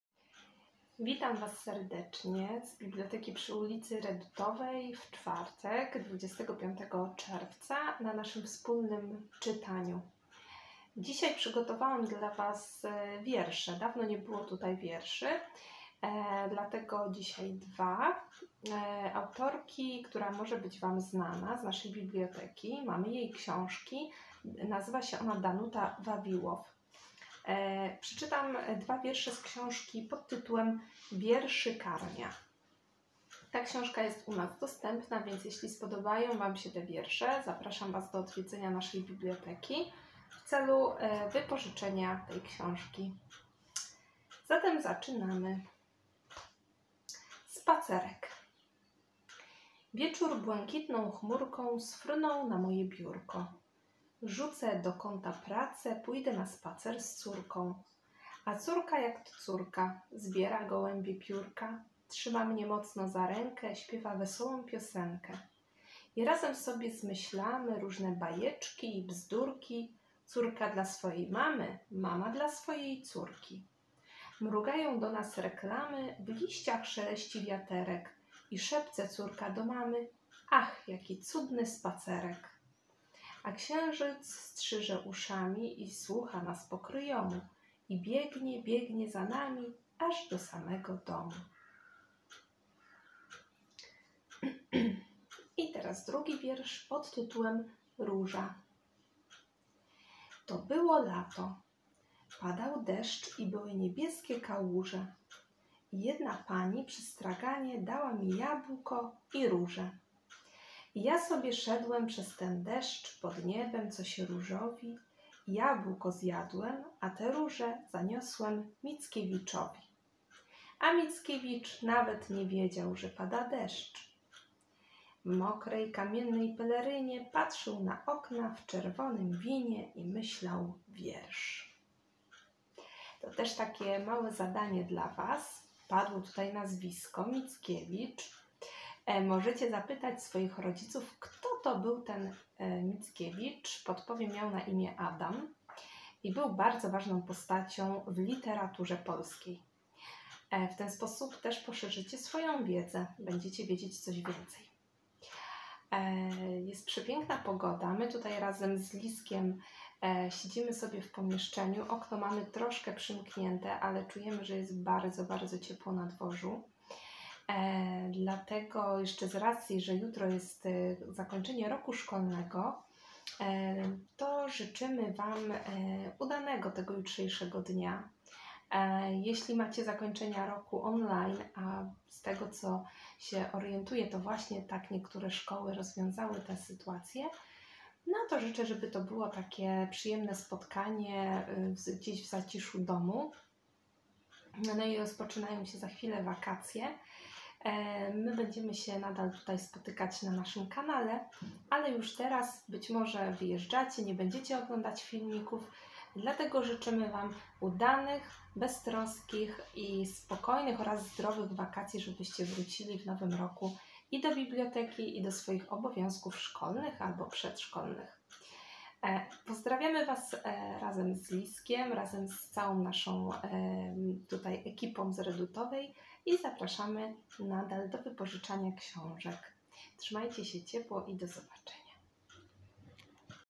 #41 Danuta Wawiłow - Wierszykarnia 🌹🌳🌲
Tydzień z filmikami kończymy dwoma wierszami Danuty Wawiłow. 😊

Do zobaczenia w poniedziałek!
A Tym, którzy wyjeżdżają na wakacje życzymy udanego i bezpiecznego wypoczynku 🤸🙌

Prezentowane teksty pochodzą z książki Danuty Wawiłow pt. "Wierszykarnia", Wydawnictwo Nasza Księgarnia.

Zabrania się kopiowania i rozpowszechniania filmu bez zgody autora.